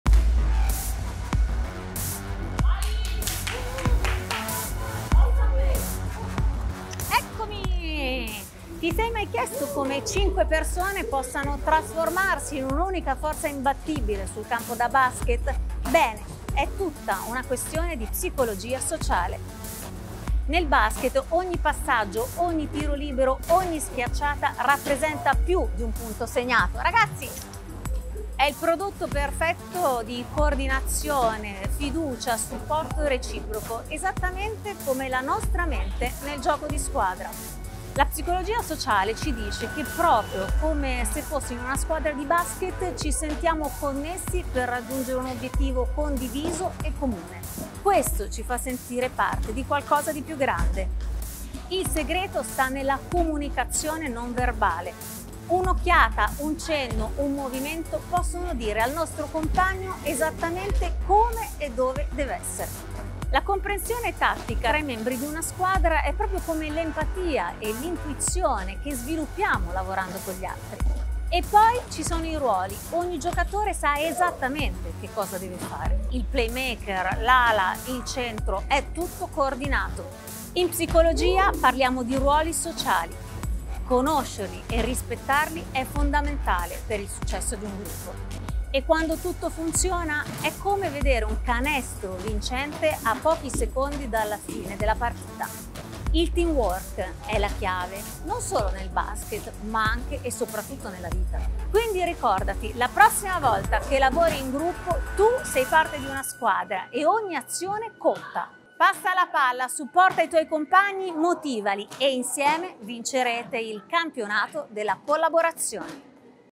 Teamwork, il potere invisibile del lavoro di squadra
Nel mondo del basket, la differenza tra una buona squadra e una grande si riduce spesso a qualcosa di cui non sempre si può sentire o parlare: la comunicazione non verbale. Allo stesso modo, nella vita, i segnali silenziosi che inviamo e riceviamo possono fare la differenza nella costruzione di relazioni e team di successo. Scopriamo l'affascinante interazione tra lavoro di squadra e segnali non verbali che non solo dominano il campo da basket, ma anche le nostre interazioni quotidiane.

Il gioco invisibile delle menti

Immaginate cinque persone su un campo da basket. A prima vista, sono solo giocatori che cercano di segnare punti. Ma guardate meglio e vedrete una conversazione complessa e silenziosa: uno sguardo qui, un gesto là. Ogni movimento è una parola, ogni giocata una frase nel linguaggio del gioco. Secondo la psicologia sociale, non si tratta solo di sport, ma di un riflesso di come noi, come persone, ci connettiamo per raggiungere obiettivi comuni. Questa connessione ci rende parte di qualcosa di più grande di noi, favorendo un senso di appartenenza e di unità.

Nella pallacanestro, ogni passaggio, tiro libero e schiacciata è più di un semplice movimento verso la vittoria; è una testimonianza della coordinazione, della fiducia e del sostegno reciproco condiviso tra i giocatori.

È un lavoro di squadra che funziona al meglio in silenzio.

La vita, come il basket, è uno sport di squadra. L’essenza dei nostri successi non risiede solo nelle capacità individuali, ma nella nostra capacità di unirci, comunicare in modo silenzioso ma efficace e perseguire obiettivi comuni con fiducia e coordinamento incrollabili.

Quindi, la prossima volta che ti ritroverai a far parte di una squadra, ricorda il potere della comunicazione non verbale e le strategie silenziose del basket. Che si tratti di un gesto di sostegno o di una mossa strategica, ogni azione che intraprendi è un elemento fondamentale per il successo collettivo.